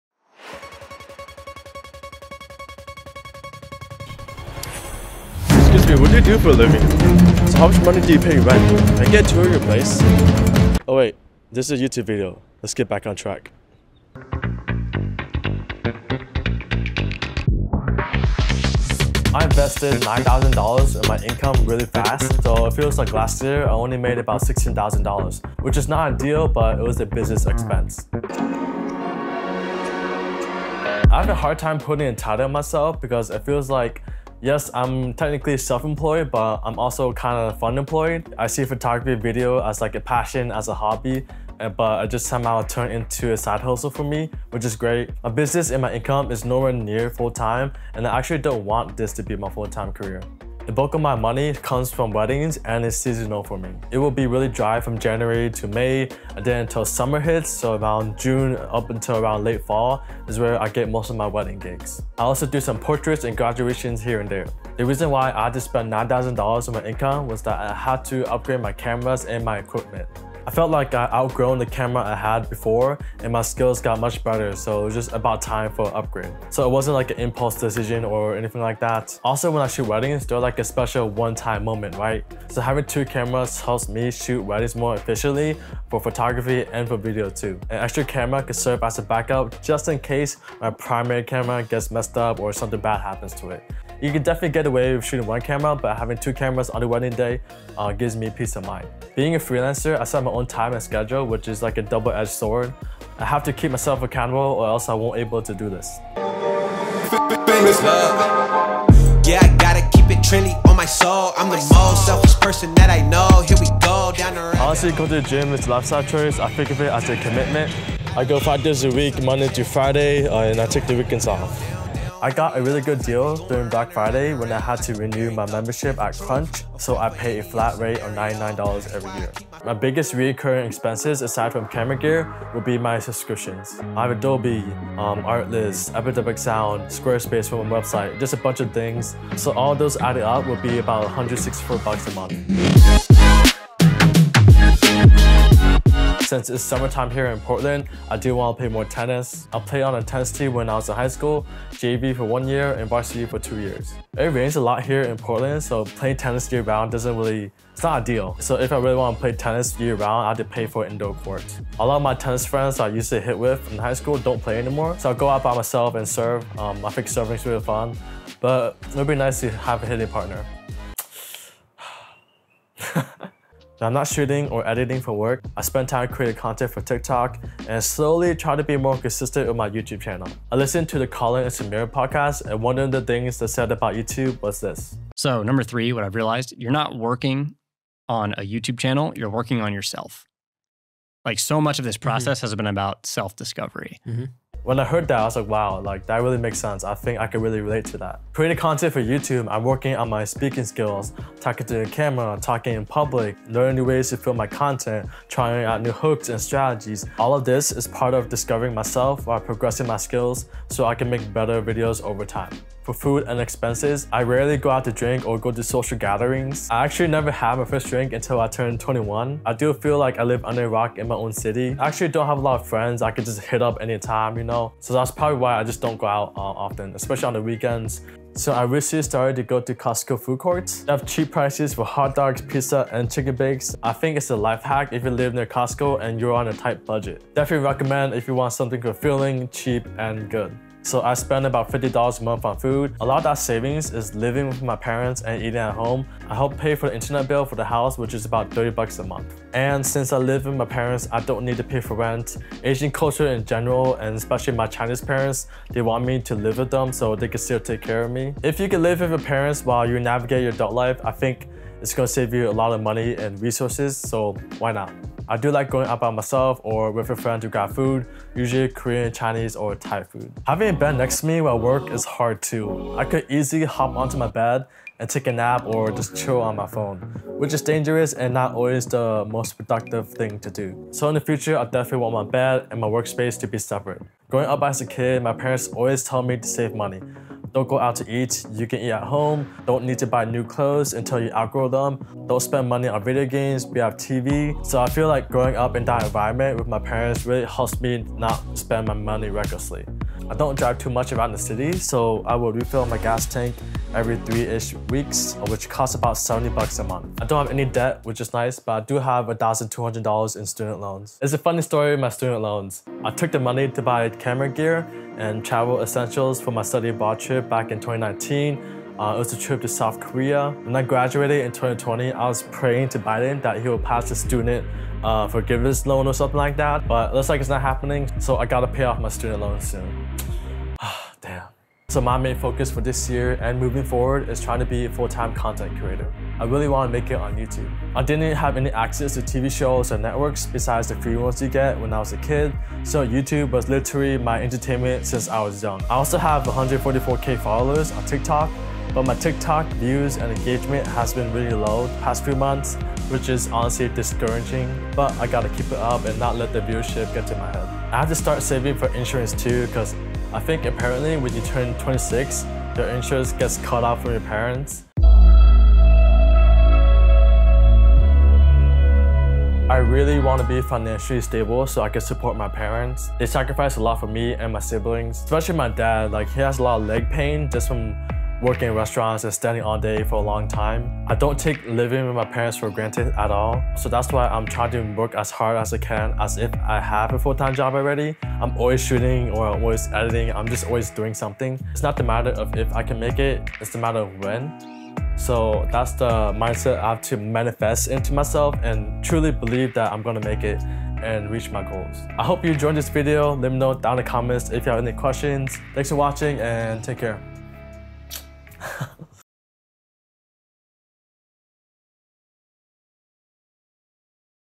Living on $25k A Year in Portland, OR | Millennial Money
I'm 25 years old and making $25,000 a year in Portland, Oregon. I work as a freelance photographer and videographer. I mainly shoot portraits and wedding-related work. When I'm not shooting for clients I work on my own social media creating content on my Tiktok and YouTube.

Main Camera #1:

Main Camera #2:

► Vlogging Camera:

► Tripod I use for everything: